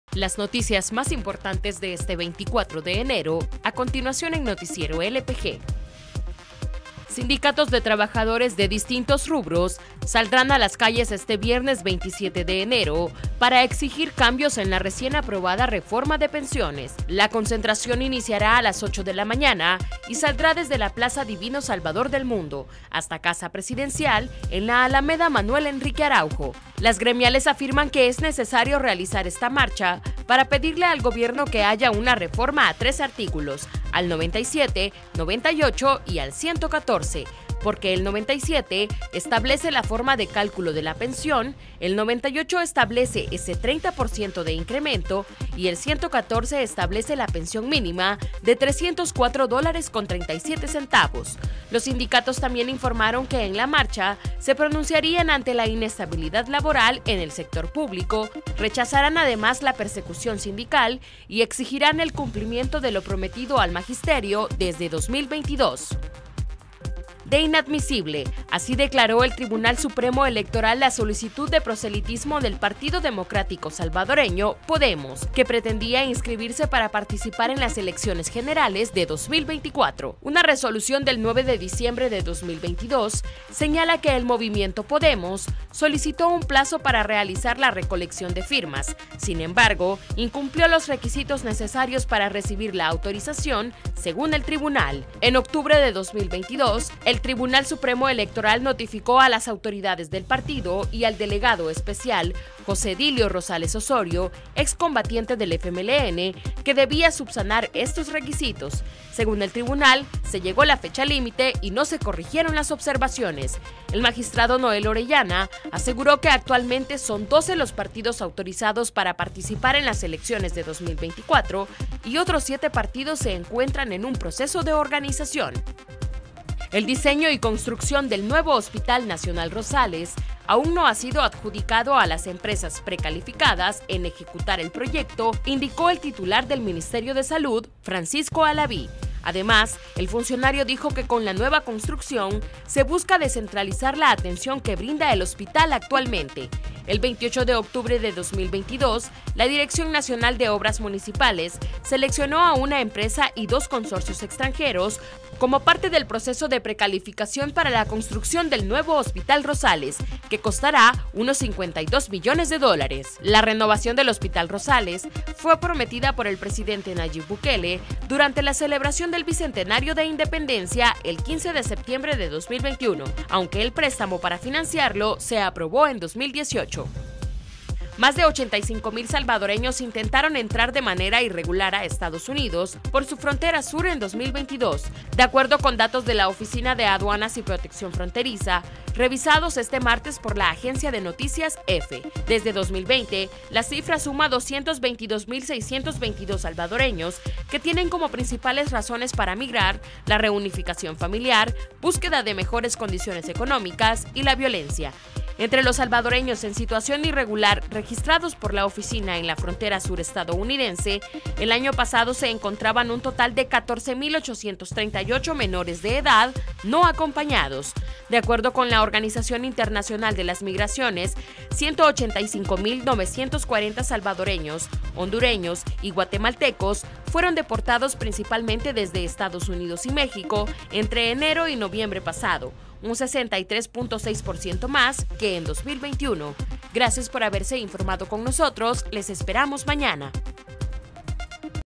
Noticiero LPG 24 de enero Sindicatos saldrán a las calles para exigir cambios a reforma de pensiones
La concentración iniciará a las 8 de la mañana y saldrá desde la Plaza Divino Salvador del Mundo hasta la Casa Presidencial, en la Alameda Manuel Enrique Araujo.